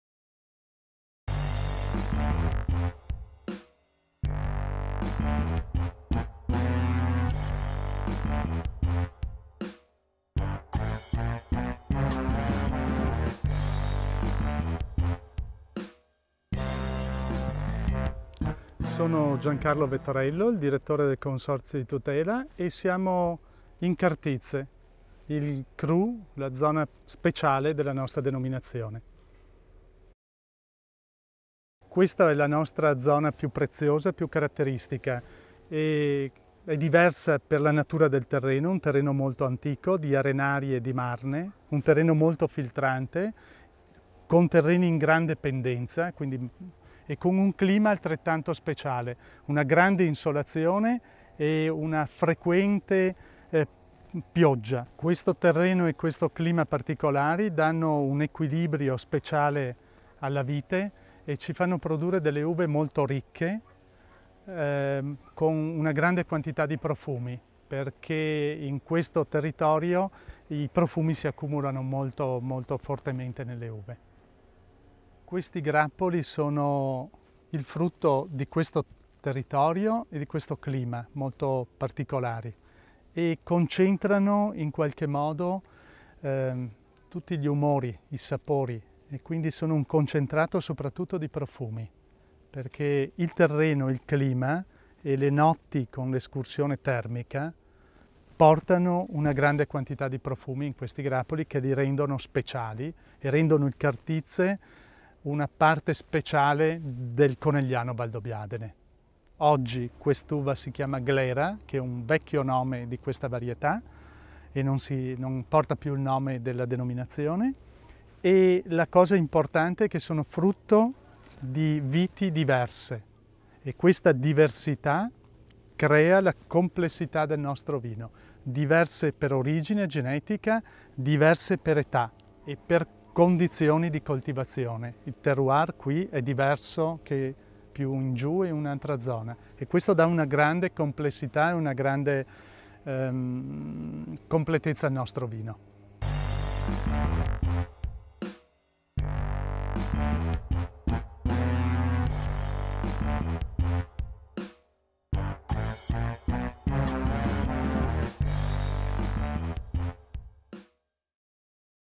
conegliano valdobbiadene by vinmedia
Giancarlo Vettorello présente le Prosecco superiore Conegliqno Valdobbiadene DOCG